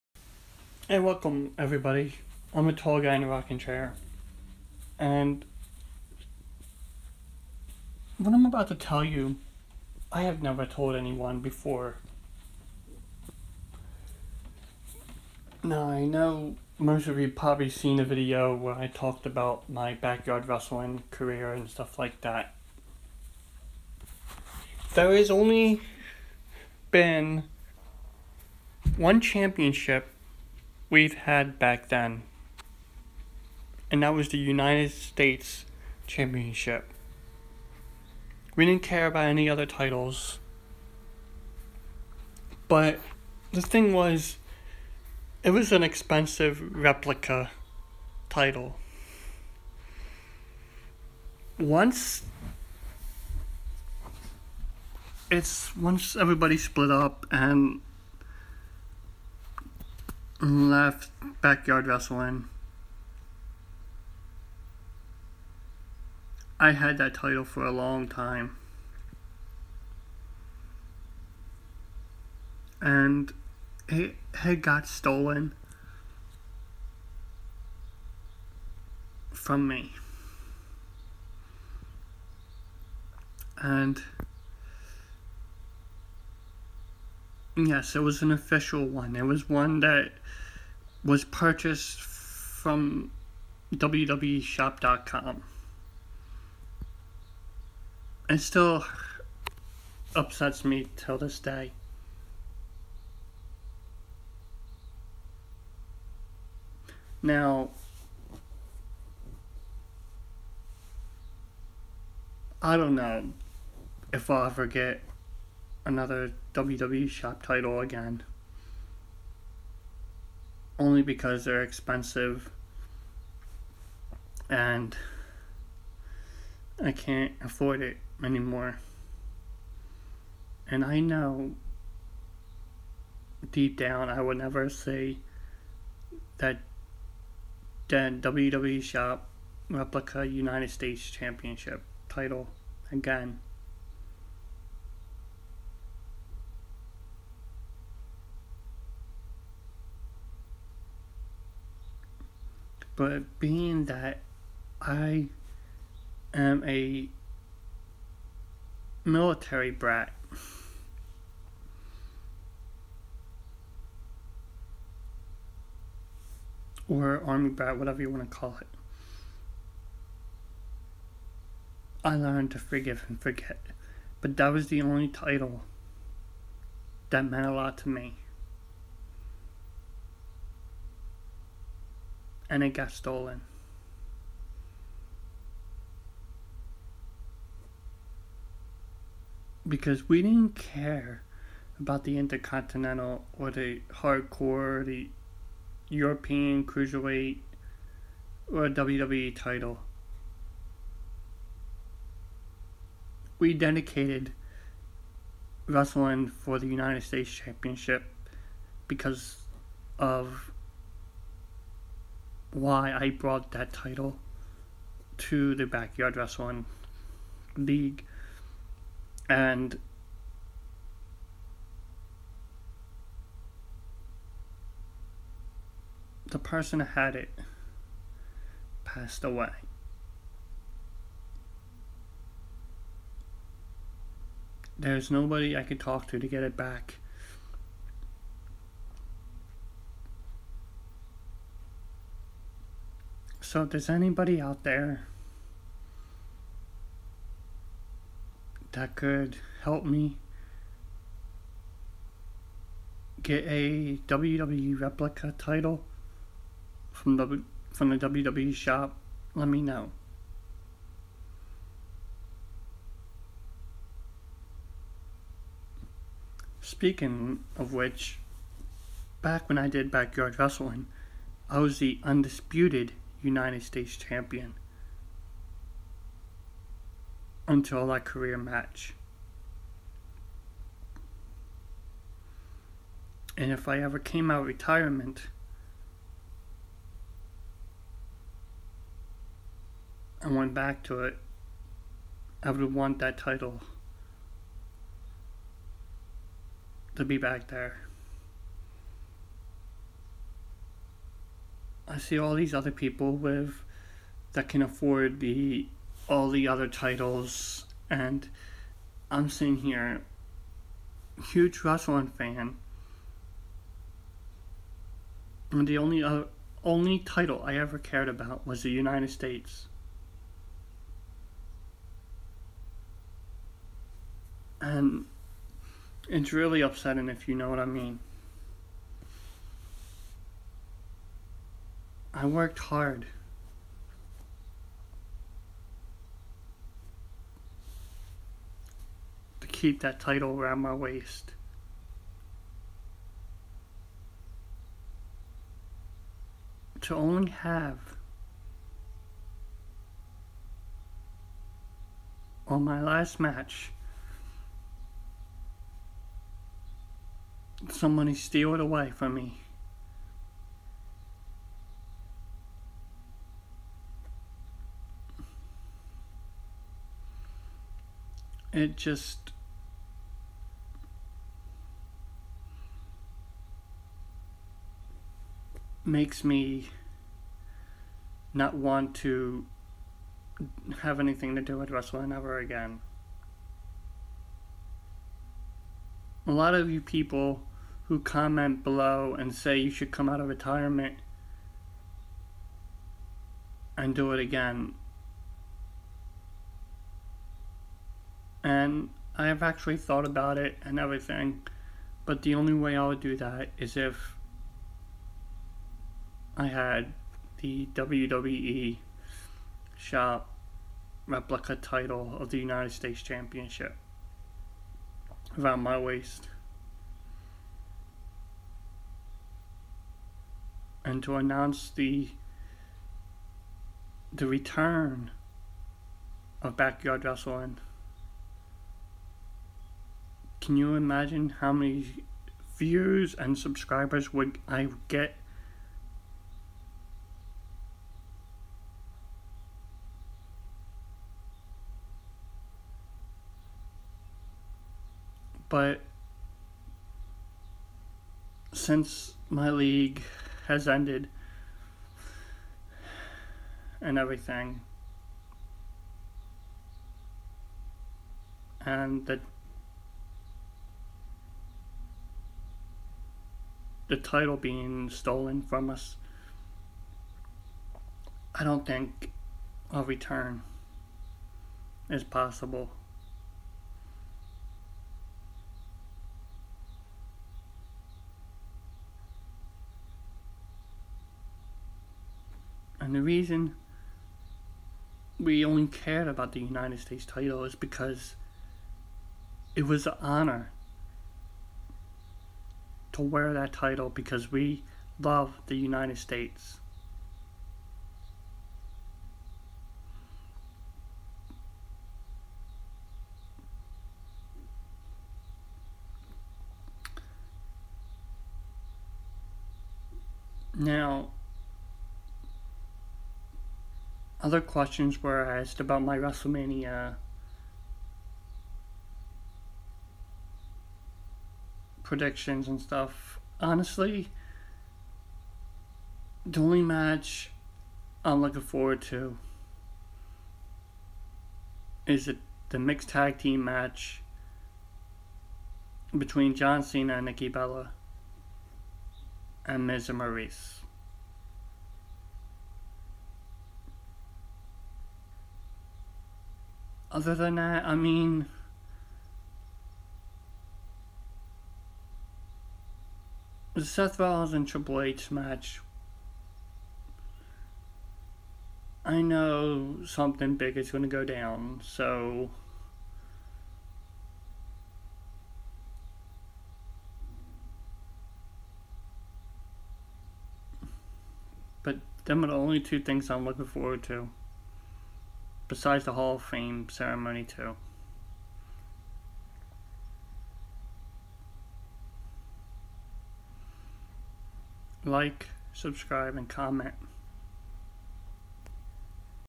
Huge update and news
Tall guys story on his wrestling and the us title

If anyone can help me with getting a WWEShop replica us title let' me know

Skype Sean Jenkins